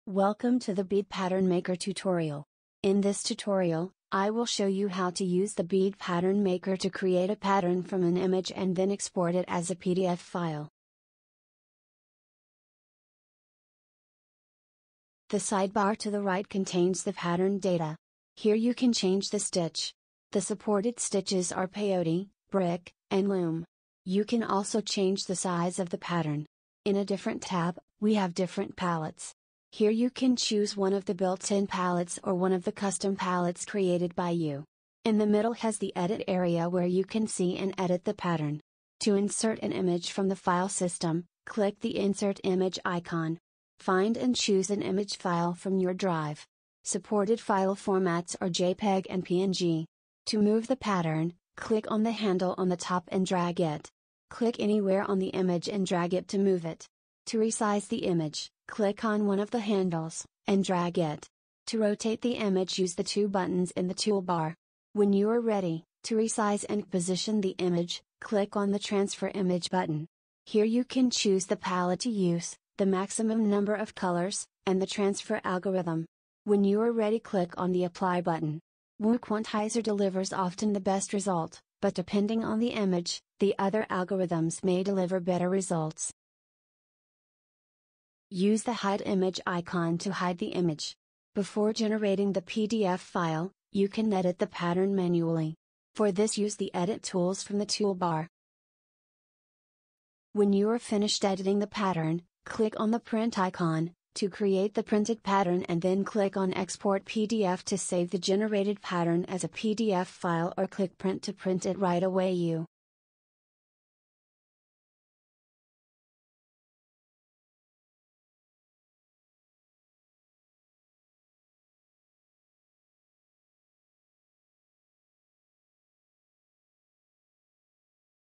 Bead Pattern Maker - Getting Started - Desktop
Powerful web tool to bring your imagination to life.
Everything you need to create a beautiful masterpiece that will make you proud.
Support Peyote, Brick, and Loom.
It works on any platform (PC, Mac, iPad, iPhone, and Android)
Image to pattern and export to PDF.